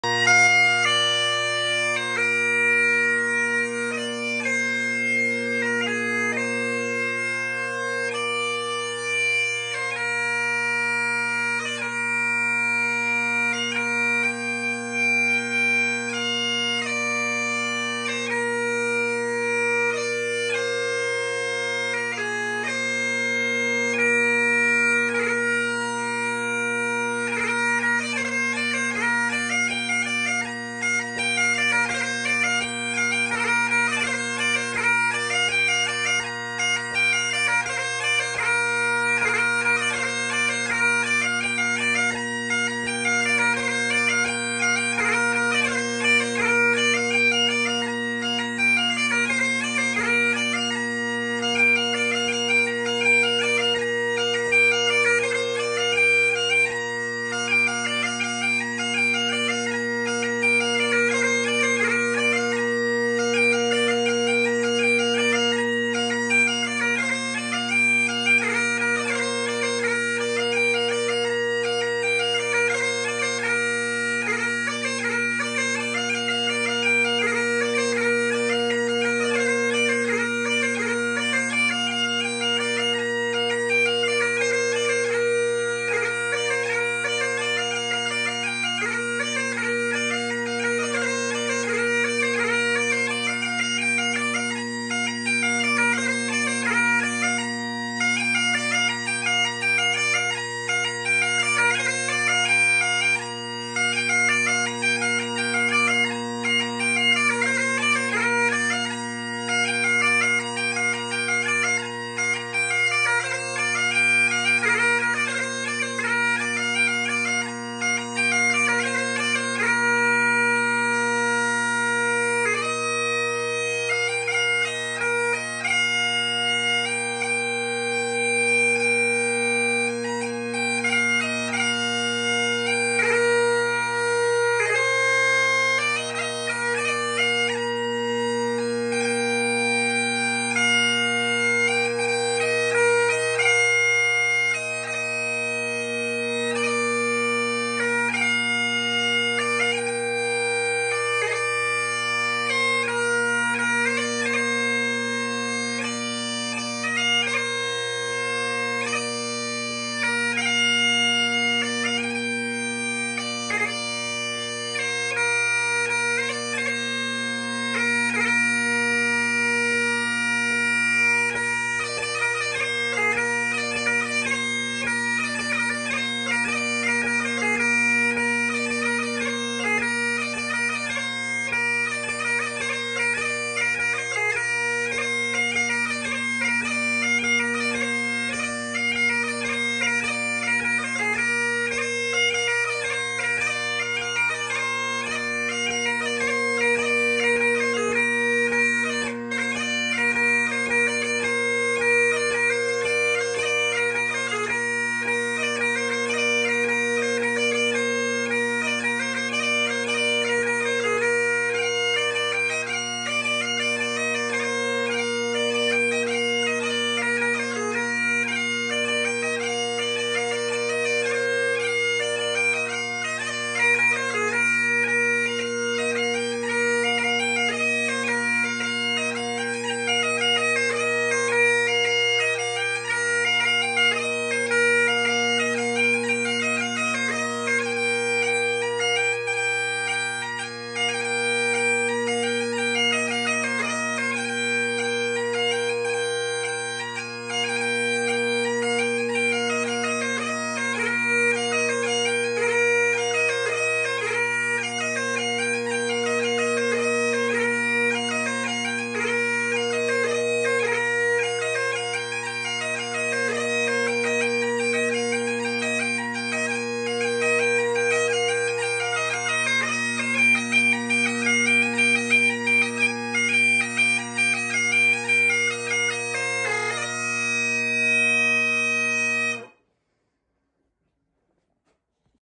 3d printed bagpipes
Just having some fun. Love how these pipes lock in easily